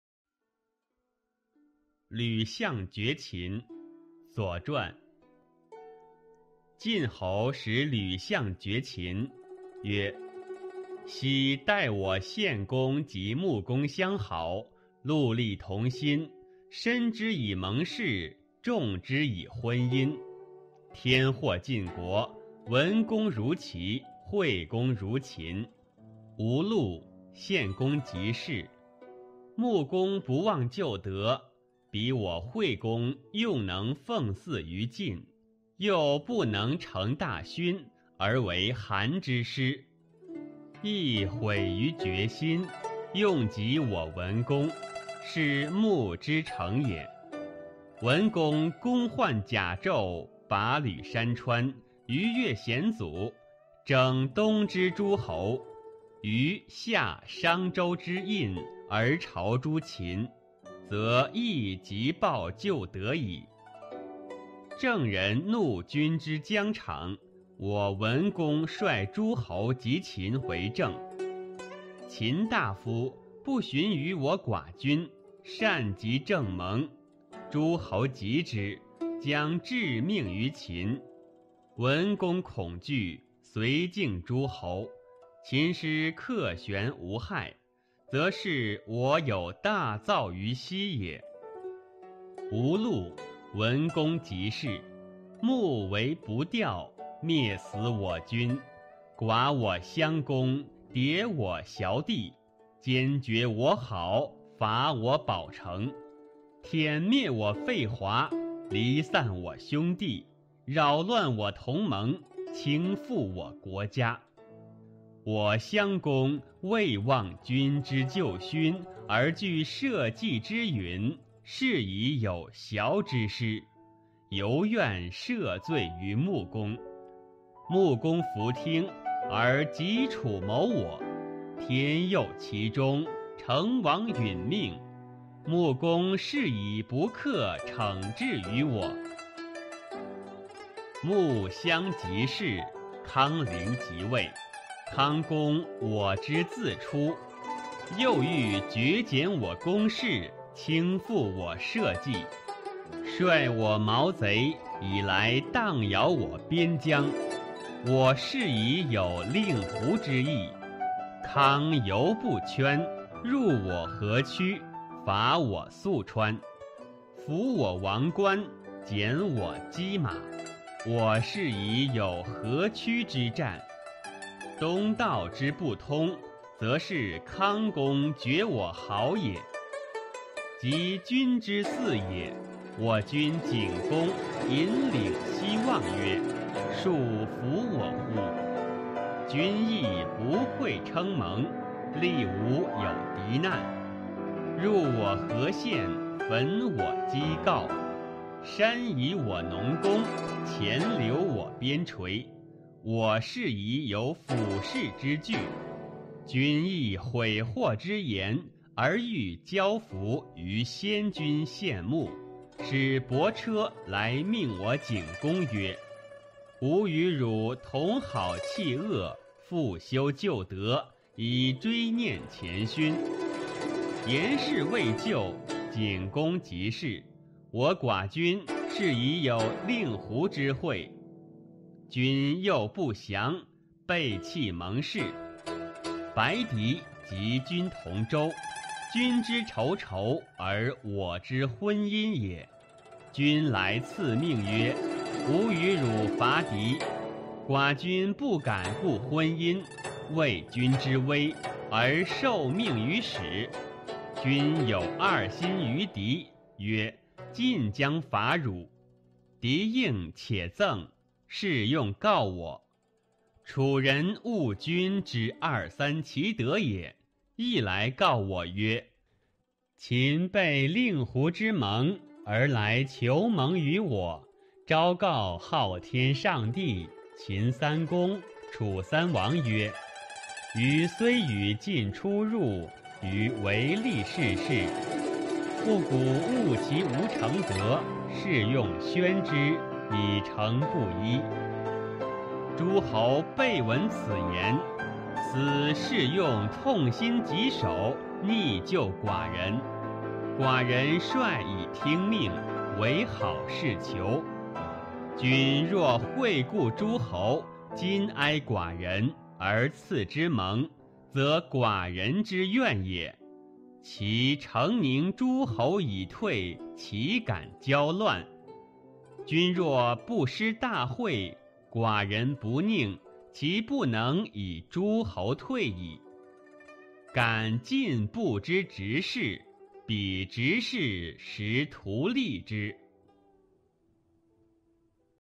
左传·吕相绝秦
晋厉公派吕相向秦国裹示绝交，说：“从前从我国的献公和贵国的穆公开始，两国相互友好，协力同心，用盟誓来申明此意，用婚姻来加强友好。上天降祸晋国，文公到了齐国，惠公到了秦国。晋国不幸，献公去世，穆公不忘旧日情谊，使我们惠公得以供奉晋国的宗庙祭祀。穆公又不能完成这一巨大功勋，而进行了韩原之战。他心里也感到后悔，因此成全了我们文公，这是穆公的成就。文公亲自披甲带盔，跋涉山川，跨越险阻，征伐东方的诸侯，使虞、夏、商、周的后裔都到秦国朝见，就已经报答旧日的恩德了。郑国人侵犯君王的边界，我们文公率领诸侯与秦国一起包围了郑国。秦国大夫不询问敝国国君，擅自与郑国订盟。诸侯痛恨这件事，要向秦国拚命。文公害怕，就安抚诸侯，秦军能够还师而安然无损，这就是我国有大功于西邻了。晋国不幸，文公去世，穆公居心不良，蔑视我们新丧故君的国家，欺我们襄公孤弱，袭击我们的殽地，断绝我们的友好，攻打我们的城堡，灭绝我们的邻邦滑国，离散我们的兄弟，扰乱我们的同盟，颠覆我们的国家。我们襄公没有忘记君王旧日的功勋，但又害怕国家的覆亡，因此有殽地之战。我们仍然希望在穆公面前求得宽恕，穆公不听，反而拉拢楚国图谋我们。上天不遂他的心意，楚成王又丧了命，穆公对我国的意图因而不能得逞。穆公、襄公去世，康公、灵公即位。康公，是我们晋国人所生的，又想削弱我们国君的宗室，颠覆我们的国家，率领我们国家的奸贼，来撼动我们的边疆，我国因此有令狐之战。康公还不改悔，又侵入我河曲一带，攻打我涑川地方，掠取我王官民众，割取我羁马土地，我国因此有河曲之战。秦国往东这条道路不通，就是由于康公断绝我们的友好的原故。

  　　等到君王继位，我们国君景公伸着脖子朝西望着说；“大概会抚恤我吧！”君王又不肯施恩举行盟会，乘我国有赤狄骚扰的患难，入侵我黄河附近各县，焚烧我箕、郜二城，抢割我庄稼，屠杀我边境百姓，我国因此有辅氏之战。君王也后悔祸患的蔓延，而想求福于两国的先君献公和穆公，派伯车来命令我们景公说：‘我与你共同友好，抛弃仇恨，重新恢复旧日情谊，以追念前人的功*(肙力)。’盟誓没有完成，景公去世，我国国君因此有令狐的盟会。君王又心怀不善，背弃盟誓。白狄与君王同在雍州，是君王的仇敌，而是我们的婚姻之国。君王派人来颁赐命令说：‘我与你一起讨伐狄人。’敝国国君不敢顾念婚姻之好，畏惧君王的威严，因而从使臣那里接受了命令。君王同时向白狄讨好，说：‘晋国要讨伐你。’狄人虽应承而内心厌恶，因此将这话告诉了我们。楚国人厌恶君王的三心二意，也来告诉我们说：‘秦国背弃令狐的盟约，而来向我国请求结盟，对着天上的上帝，秦国去世的三公、楚国去世的三王明白宣告：他虽然与晋国往来，他只看自己的利益。我厌恶他没有始终如一的品德，因此把这些话公布出来，以警戒不专一的人。’诸侯都听到了这话，因而痛恨之至，都来亲近敝国国君。敝国国君率领他们前来听命，只求和好。君王如果施恩照顾诸侯，哀怜敝国国君，而赐绐我们盟约，就是敝国国君的愿望。敝国国君将会安抚诸侯退兵，岂敢谋求战乱？君王如果不肯施大恩，敝国国君无能，也就不能率领诸侯退兵了。冒昧地将这些话全部陈述于君王的左右，以便君王的左右从有利方面认真加以考虑。”